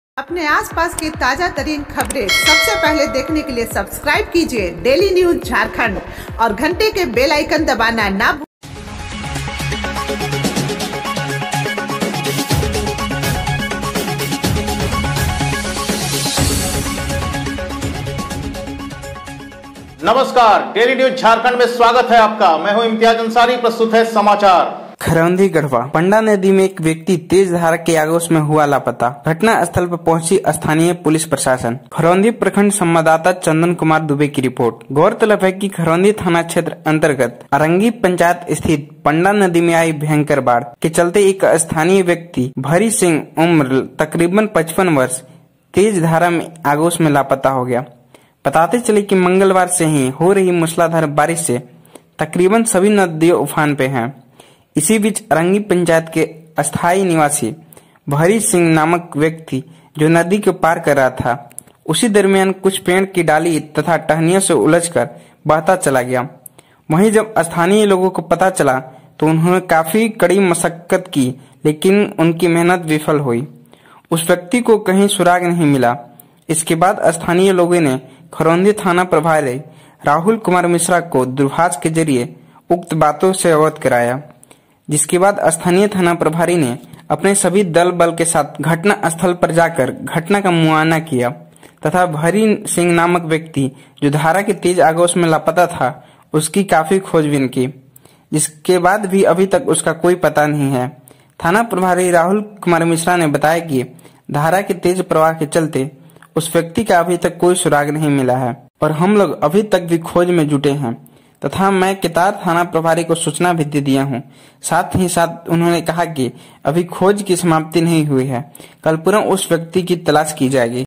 Garhwa ।। नदी में एक व्यक्ति तेज धारा की आगोश में हुआ लापता।
💢 खरौंधी (गढ़वा):-  पंडा नदी में एक व्यक्ति तेज धारा की आगोश में हुआ लापता।
घटनास्थल पर पहुंची स्थानीय पुलिस प्रशासन।

👉👉 खरौंधी प्रखंड संवाददाता चंदन कुमार दुबे की रिपोर्ट:-

   गौरतलब है कि खरौंधी थाना क्षेत्र अंतर्गत अरंगी पंचायत स्थित पंडा नदी में आई भयंकर बाढ़ के चलते एक स्थानीय व्यक्ति भरी सिंह उम्र तकरीबन 55 वर्ष धारा की तेज़ आगोश में लापता हो गया। बताते चलें कि मंगलवार से हीं हो रही मूसलाधार बारिश से तकरीबन सभी नदियां उफान पर हैं।  इसी बीच अरंगी पंचायत के स्थाई निवासी भरी सिंह नामक व्यक्ति जो नदी को पार कर रहा था,  उसी दरमियान कुछ पेड़ की डाली तथा टहनियों में उलझ कर बहता चला गया ,  वहीं जब स्थानीय लोगों को पता चला तो उन्होंने काफी कड़ी मशक्कत की लेकिन उनकी मेहनत विफल हुई,  उक्त व्यक्ति का कहीं सुराग नहीं मिला।   इसके बाद स्थानीय लोगों ने खरौंधी थाना प्रभारी राहुल कुमार मिश्रा को दूरभाष के जरिए उक्त बातों से अवगत कराया जिसके बाद स्थानीय थाना प्रभारी ने अपने सभी दल बल के साथ घटनास्थल पर जाकर घटना का मुआयना किया , तथा भरी सिंह नामक व्यक्ति जो धारा की तेज आगोश में लापता था उसकी काफी खोजबीन की गई , जिसके बाद भी अभी तक उसका कोई पता नहीं है। थाना प्रभारी राहुल कुमार मिश्रा ने बताया  कि धारा की तेज प्रवाह के चलते उक्त व्यक्ति का अभी तक कोई सुराग नहीं मिला है और हम लोग अभी तक भी खोज में में जुटे हैं तथा मैं केतार थाना प्रभारी को सूचना भी दे दिया हूं। साथ ही साथ उन्होंने कहा के अभी खोज भी समाप्त नहीं हुई है कल पुनः उक्त व्यक्ति की तलाश की जाएगी।
मौके पर  स्थानीय ग्रामीण एवं सभी पुलिस कर्मी मौजूद थे।